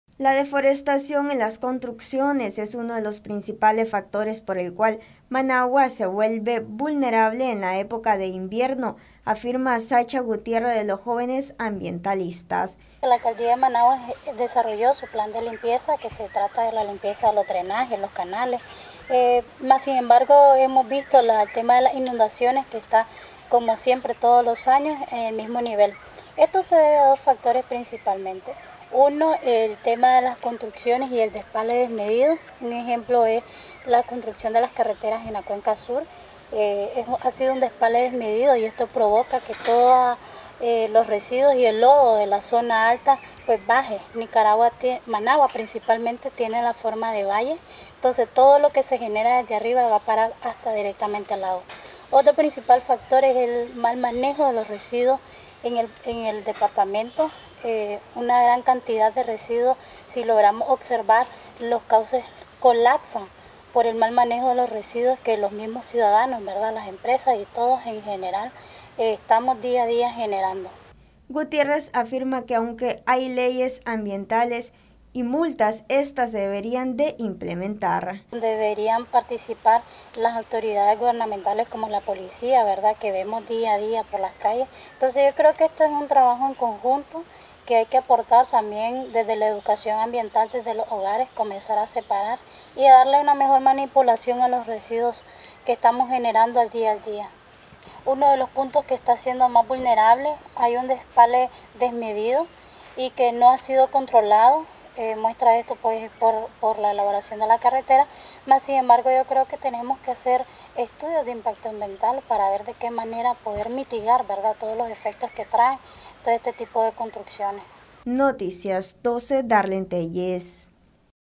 “Mal manejo de residuos uno de los tantos factores que provoca vulnerabilidad con las lluvias”
Deforestación y construcción   uno de los factores  que hace que Managua sea vulnerable a las lluvias, afirma Zacha Gutiérrez de Jóvenes Ambientalistas.